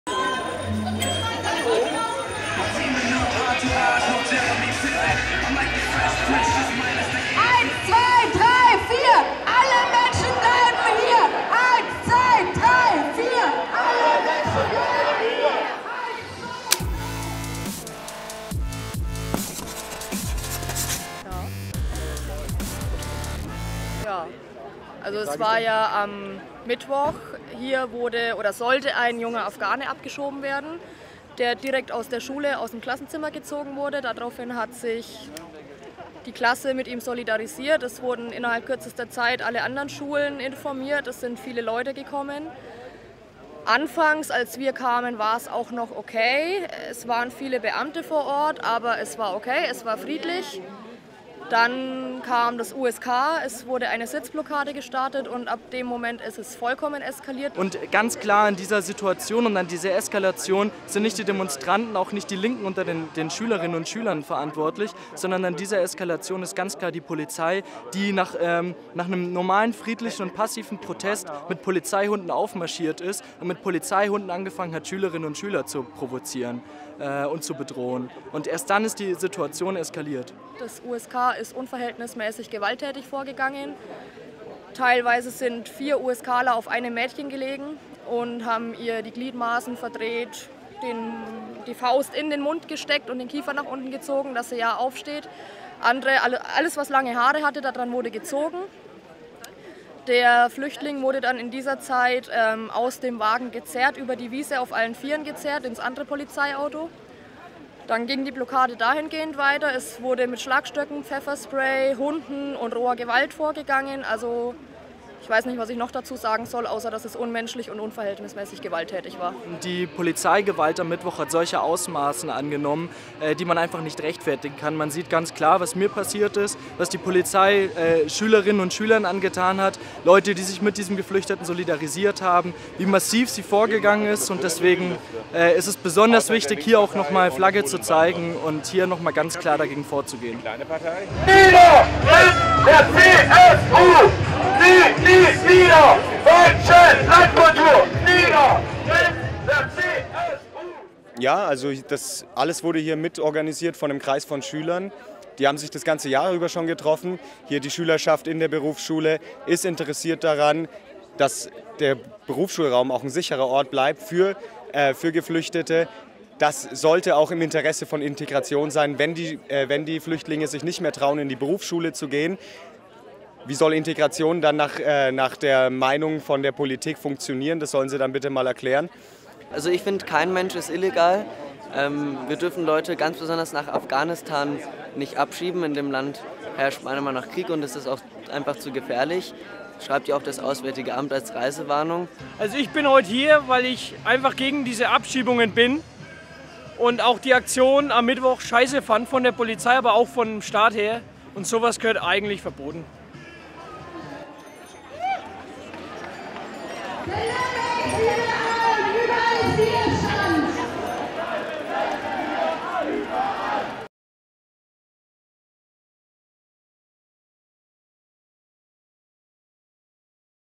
eNgage - Demonstration gegen Abschiebung
Nach den Vorkommnissen am 31.05. haben sich Abschiebungsgegner am 02.06. am Berliner Platz zu einer Demonstration zusammengefunden. Wir waren unterwegs und haben ein paar Impressionen eingefangen.

Ihr seid aus Nürnberg und habt Lust bei CiTyVee mitzuproduzieren ?